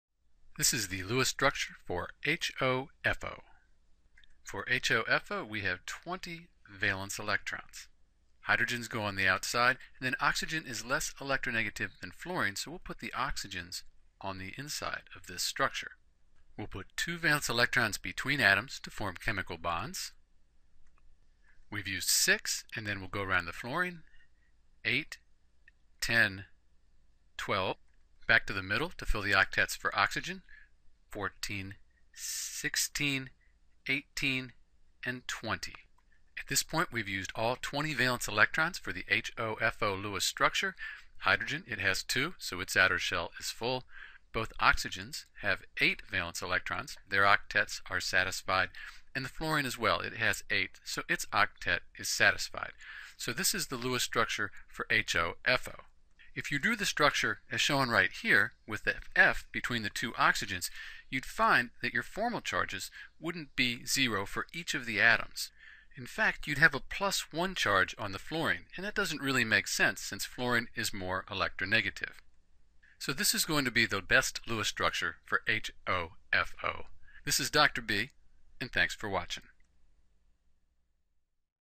HOFO Lewis Structure: How to Draw the Lewis Structure for HOFO
A step-by-step explanation of how to draw the HOFO Lewis Dot Structure.

For the HOFO structure use the periodic table to find the total number of valence electrons for the HOFO molecule. Once we know how many valence electrons there are in HOFO we can distribute them around the central atom with the goal of filling the outer shells of each atom.

In the Lewis structure of HOFO structure there are a total of 20 valence electrons.

Steps to Write Lewis Structure for compounds like HOFO
1. Find the total valence electrons for the HOFO molecule.
2. Put the least electronegative atom in the center. Note: Hydrogen (H) always goes outside.
3. Put two electrons between atoms to form a chemical bond.
4. Complete octets on outside atoms.
5. If central atom does not have an octet, move electrons from outer atoms to form double or triple bonds.